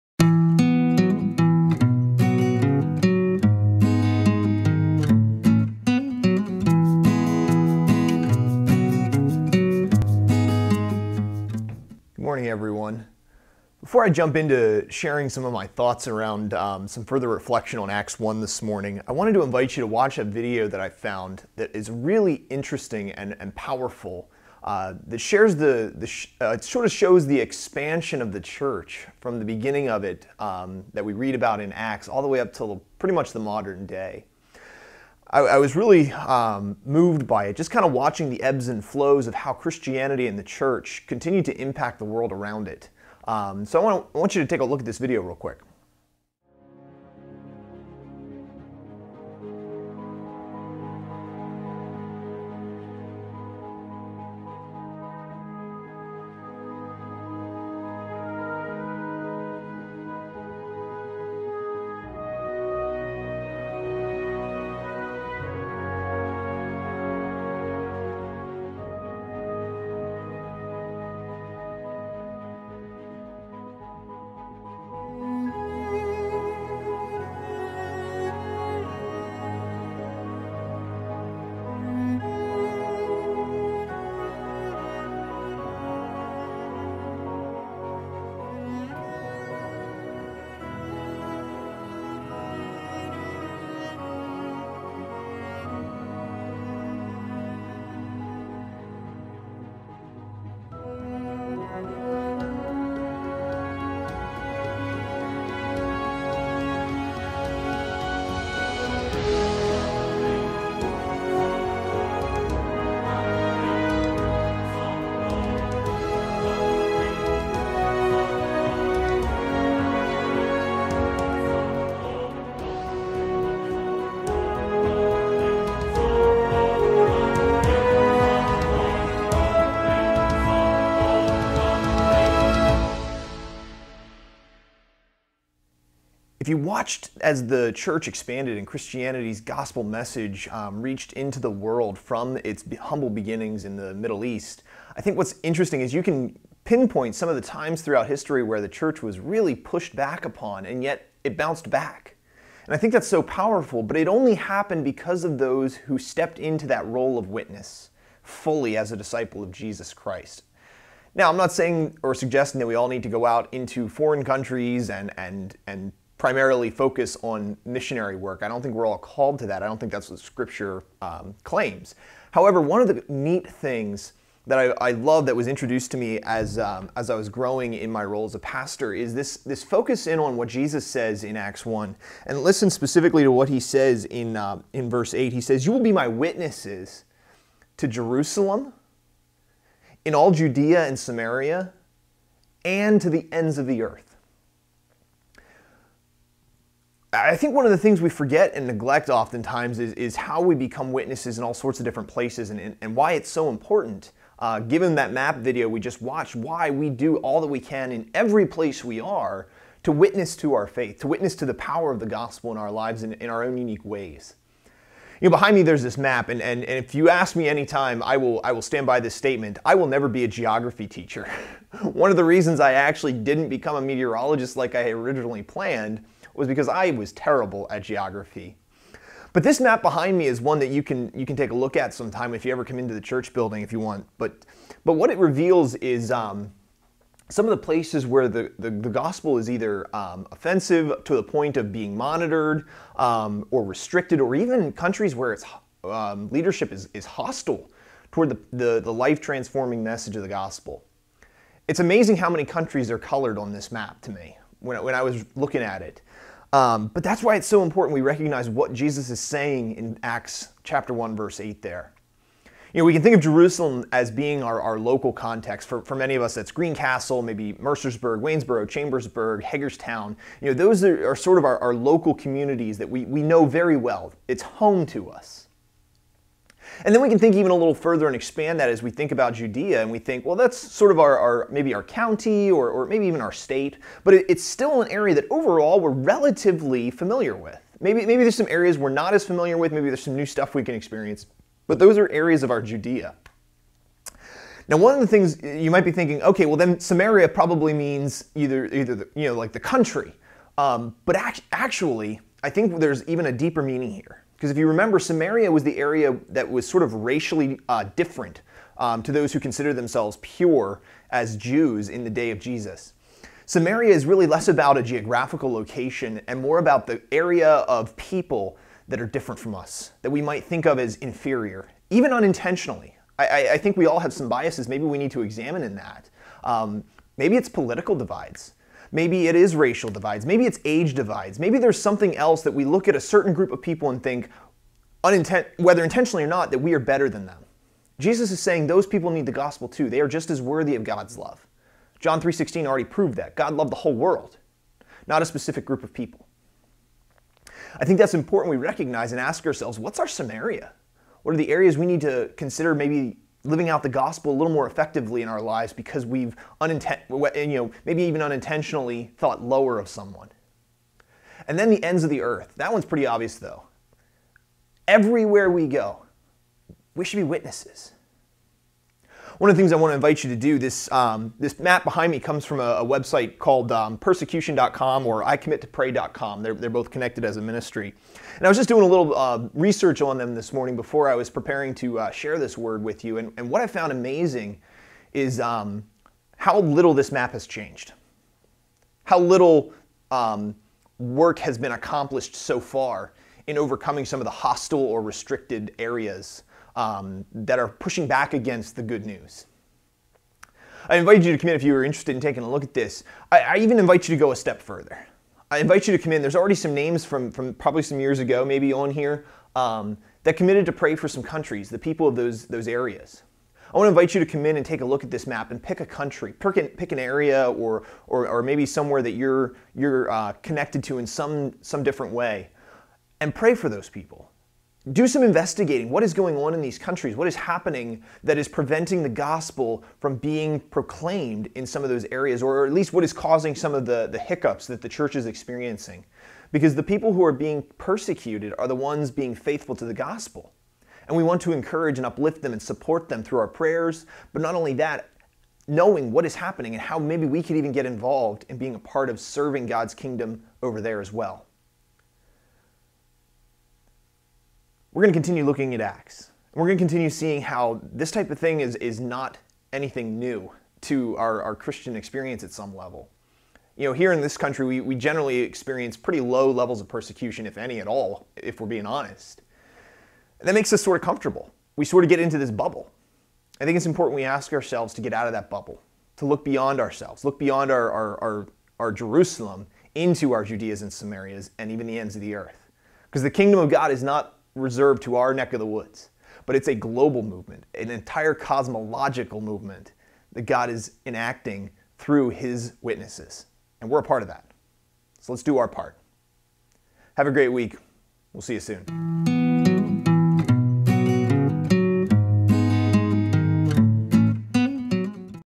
Coffee Break w/ Pastor Ryan - Witness
4/14/2021
When Jesus told his followers that they would be witnesses to his acts, teachings, and resurrection, they were told where they would be going to share that good news. Hint: it was everywhere! This week, Pastor Ryan unpacks Acts 1:8 even further as it is so central to understanding not only the book of Acts, but also our calling to live as disciples and witnesses to Jesus Christ today.